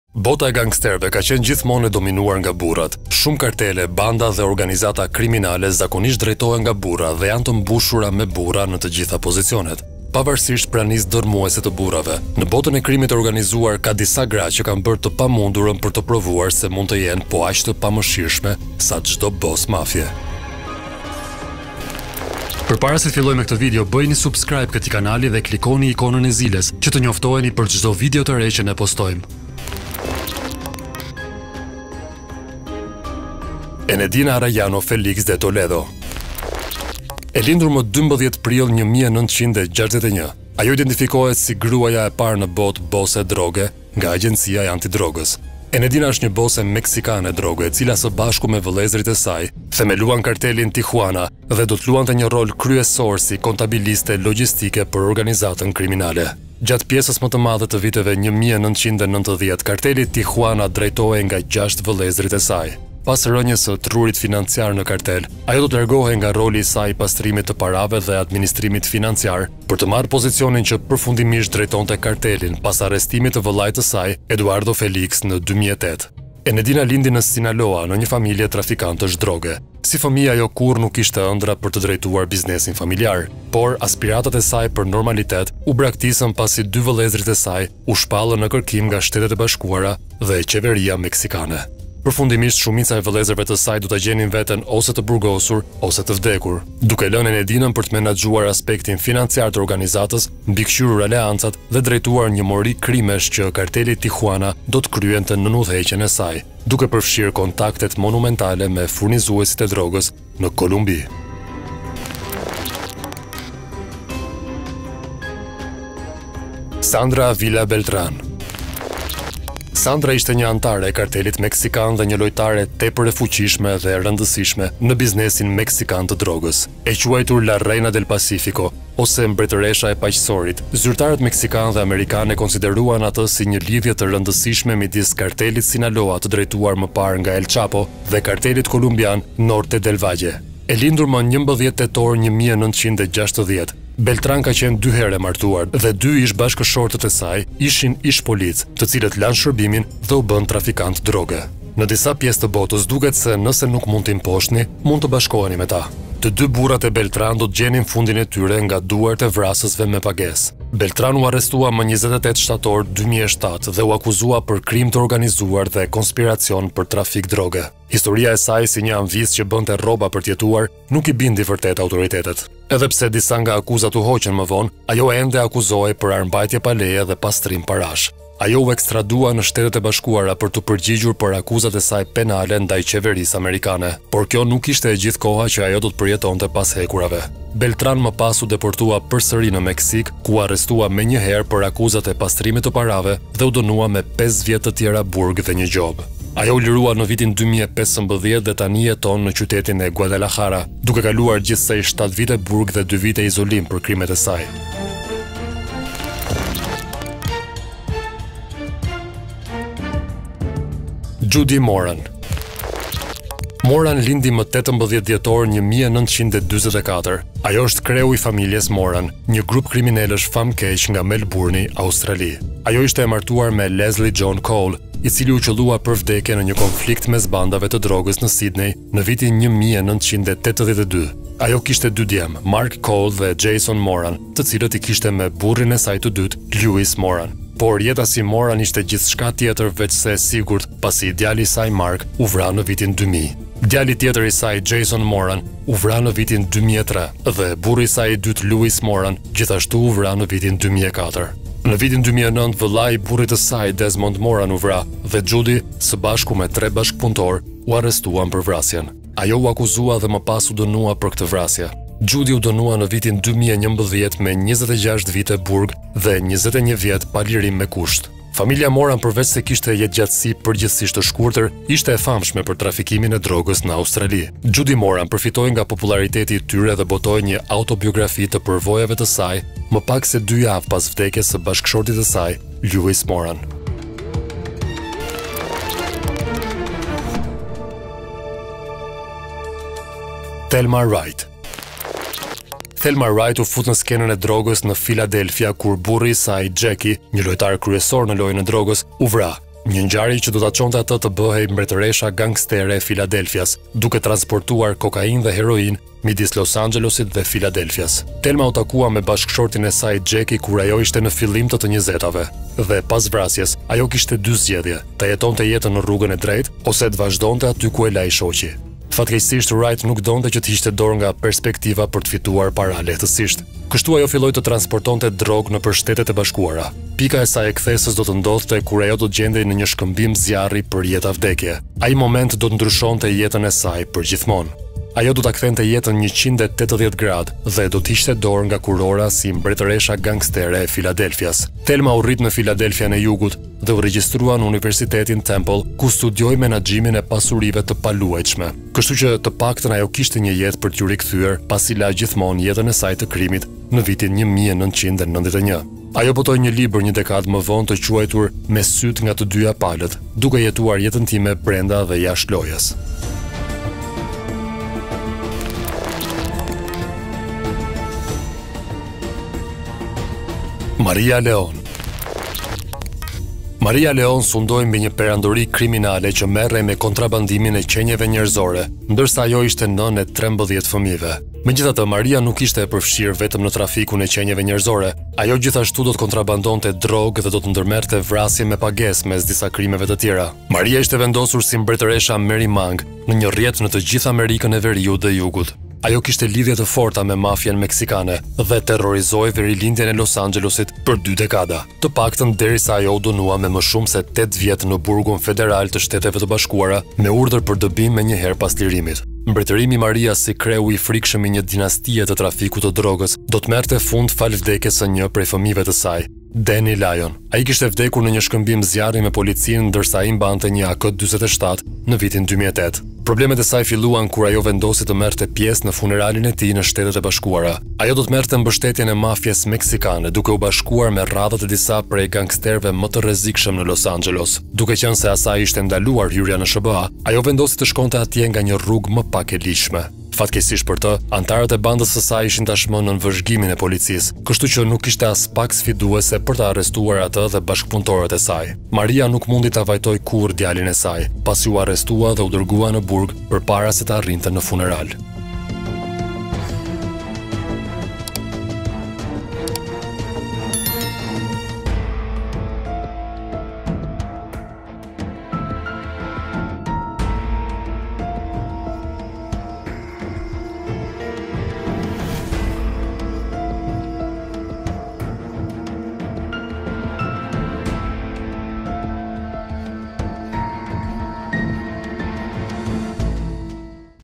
Femrat Gangstere me te Rrezikshme ne Bote • Fakte Interesante
E lindur më 12 prill 1961 ajo identifikohet si gruaja e parë në botë bose droge nga Agjencia e antidrogës.
Enedina është një bose meksikane droge, e cila së bashku me vëllezërit e saj themeluan kartelin Tijuana dhe do të luante një rol kryesor si kontabiliste logjistike për organizatën kriminale.
Gjatë pjesës më të madhe të viteve 1990 karteli Tijuana drejtohej nga gjashtë vëllezërit e saj.
Pas rënies së trurit financiar në kartel, ajo do të largohej nga roli i saj i pastrimit të parave dhe administrimit financiar për të marrë pozicionin që përfundimisht drejtonte kartelin pas arrestimit të vëllait të saj Eduardo Felix, në 2008.
Enedina lindi në Sinaloa në një familje trafikantësh droge. Si fëmijë, ajo kurrë nuk kishte ëndrra për të drejtuar biznesin familjar.